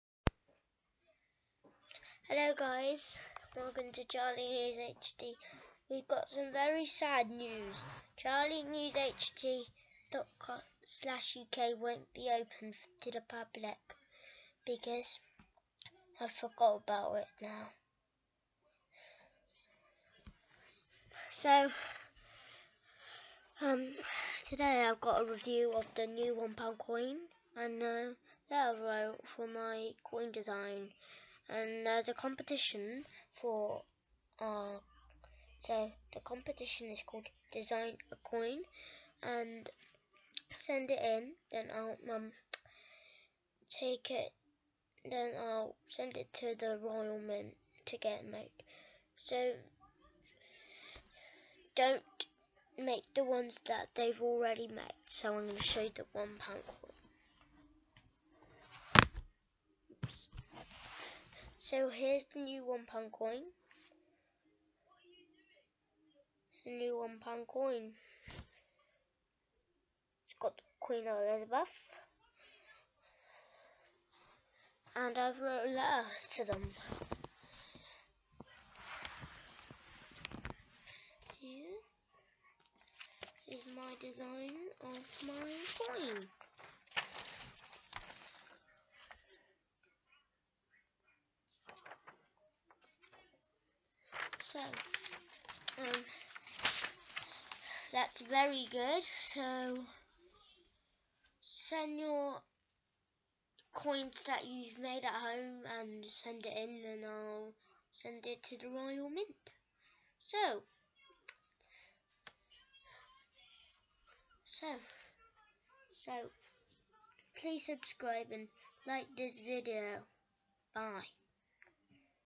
special edition royal mint video brodcased by charlie news hd
first we have some sad news Charlie's website is closed. next it is royal mint letter with a design under it. of course the new one pound coin was realesed on the 28th march 2017. well done royal mint for your hard work for making all the coins you had made